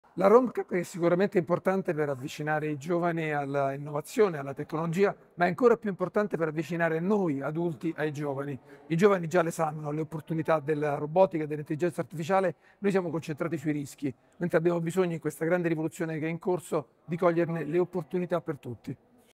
IA: cogliere le opportunità per tutti
La RomeCup è importante anche per noi adulti, ancora incentrati sui rischi dell'IA, quando serve coglierne le opportunità per tutti.

Riccardo Luna, conduttore e direttore di Italian Tech.